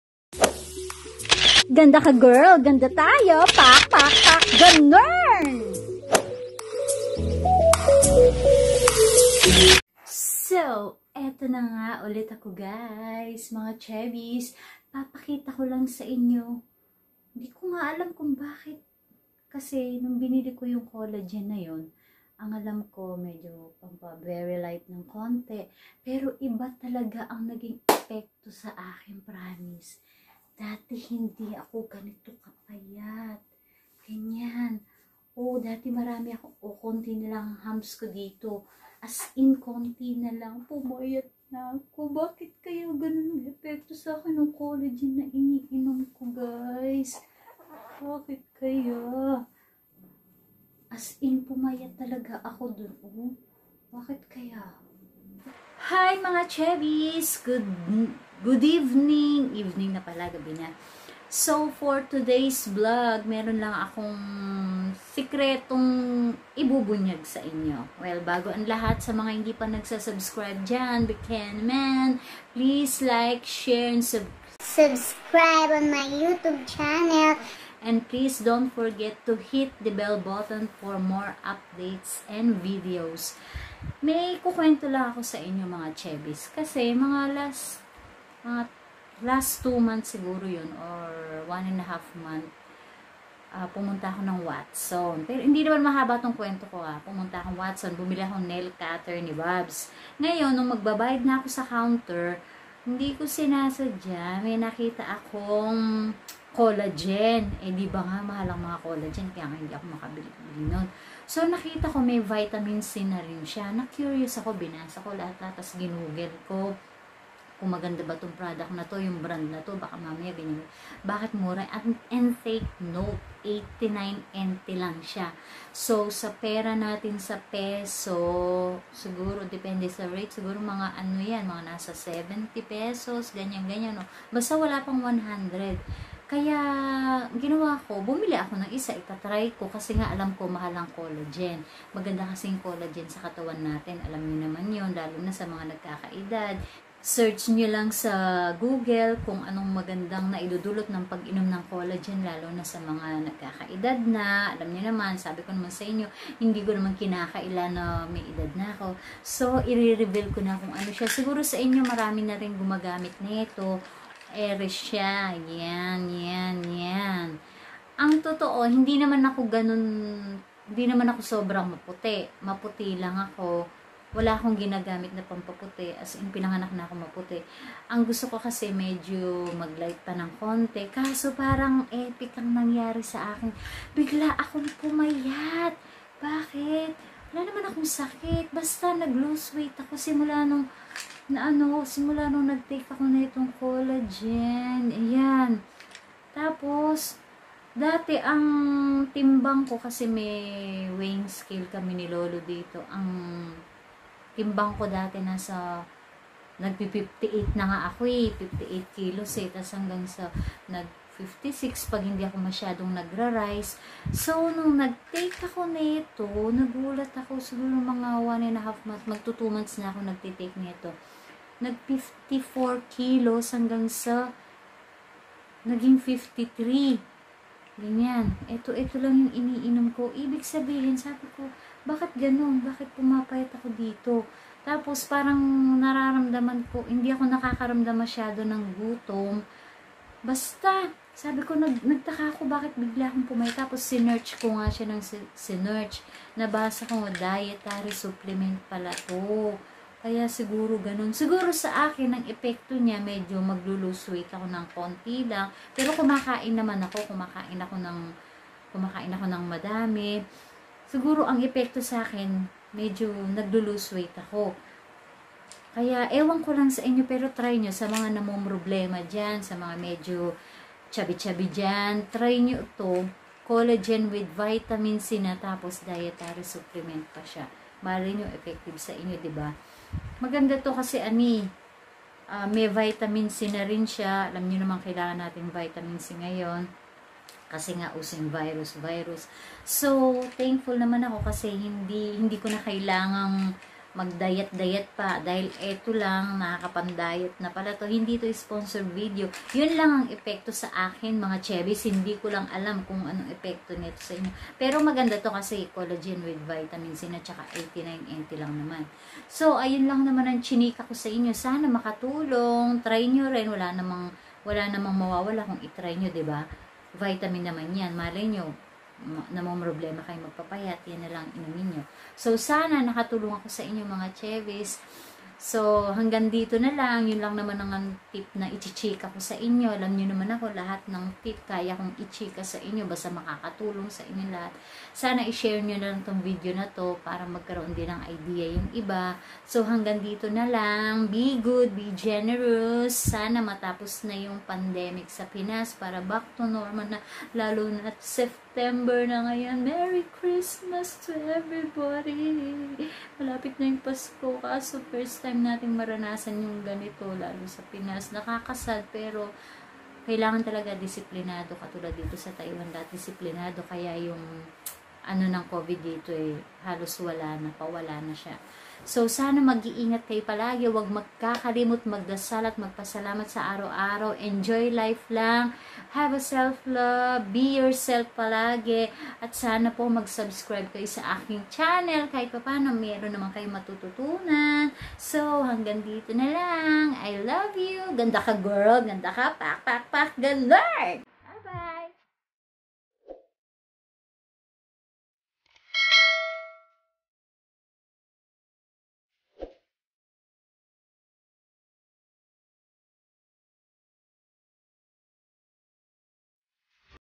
PAANO BA AKO PUMAYAT#VLOG20
Sa video na ito ipapaliwanag ko kung paano nga ba ako bigla nag lose weight..
COLLAGEN WITH VIT.C BY NUTRIPLUS for almost 2months ko pag inom nito bigla ako pumayat...2 tablets per day and nabili ko lang ito sa watson ng NTD89